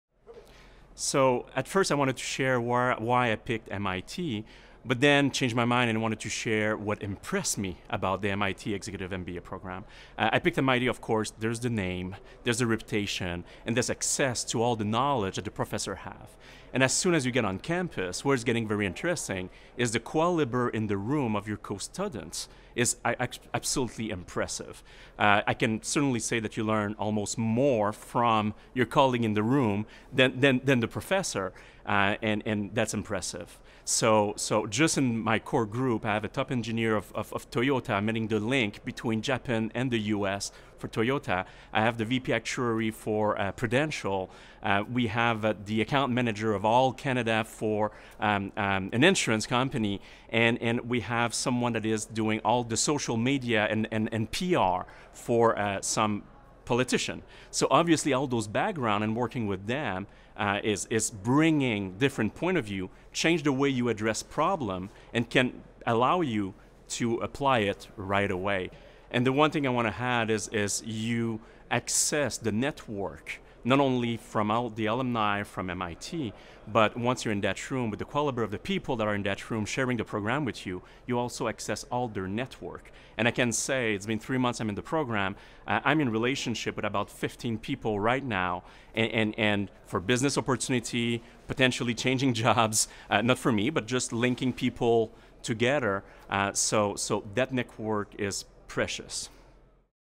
The MIT Executive MBA: Building a Network for the Future
Sebastien Girard, vice president of talent acquisition and workforce planning at Parkland Hospital, reflects on the factors that influenced his decision to pursue the MIT Executive MBA, including the school and program’s reputation access to knowledgeable professors, and the caliber of his fellow student colleagues. “You can almost learn more from your colleagues in the room than the professor—and that’s impressive,” Girard says.
MIT’s Executive MBA program brings together individuals who are extraordinarily talented and driven to learn from the program, and each other. Current student Sebastien Girard’s classmates included a global sales director for Toyota, a vice president for Prudential, a Director for Canada’s largest instrument manufacturer, and the director of social media for a political candidate. Participants not only have access to their colleagues’ insights and ideas during the program, but for the duration of their professional careers.